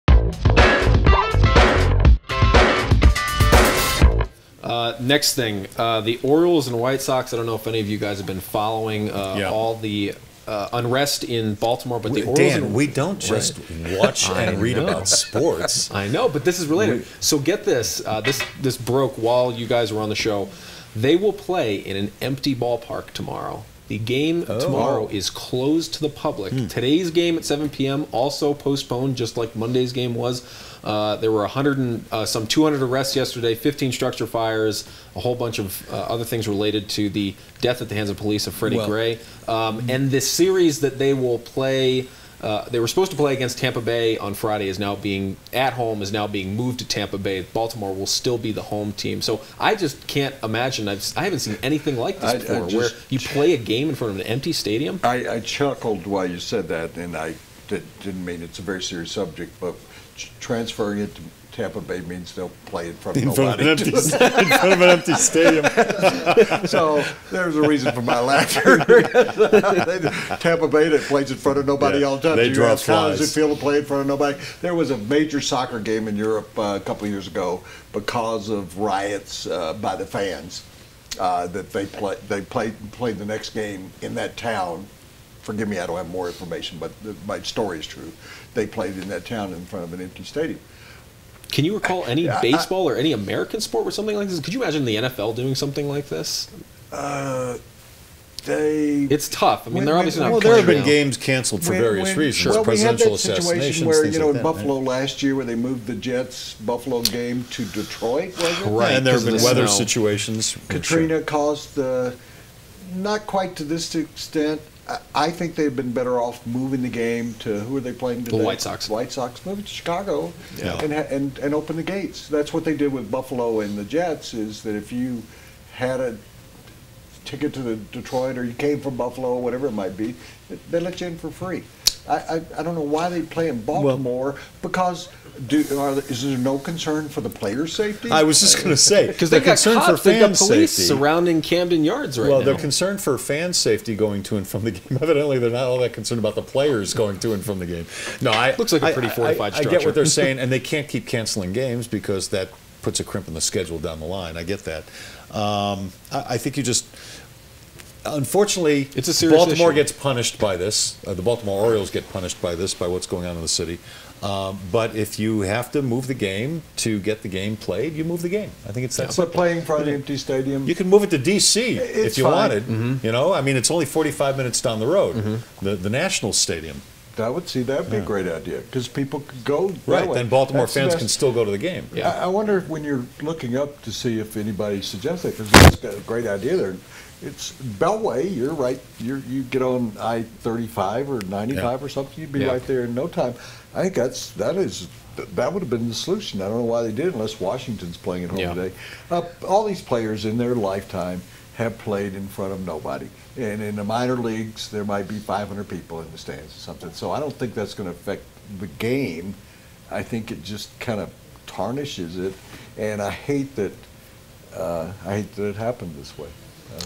Trending Now: Wednesday's Baltimore Orioles vs Chicago White Sox game will be closed to the public
The Denver Post's Dan Petty joins The Sports Show to discuss the hottest topics inside the world of sports on April 28, 2015.